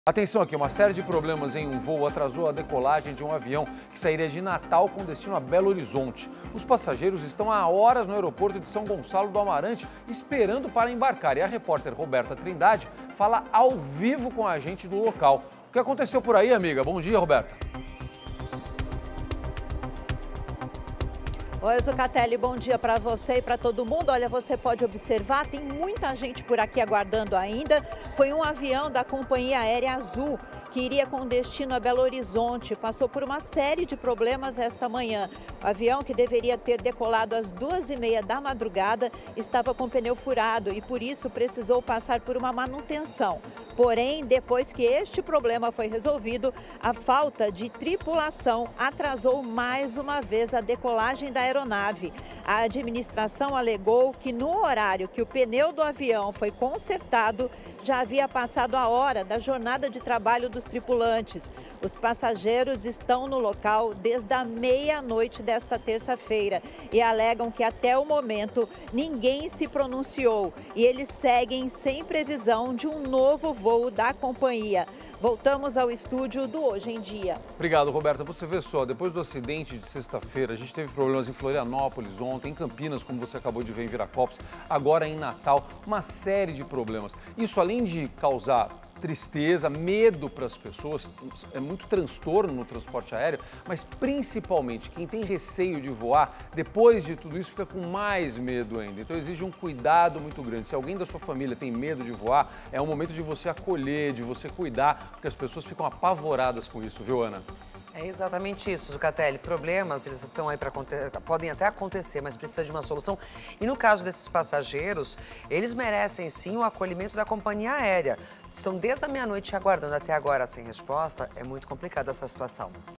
Problemas em voo de Natal a Belo Horizonte atrasam decolagem
Um voo com destino a Belo Horizonte (MG) enfrentou problemas que resultaram no atraso da decolagem. O avião deveria ter partido às duas e meia da madrugada do aeroporto de São Gonçalo do Amarante, no Rio Grande do Norte. Inicialmente, o atraso foi causado por um pneu furado que necessitou de manutenção. Após o conserto do pneu, uma nova demora ocorreu devido à falta de tripulação disponível para operar o voo.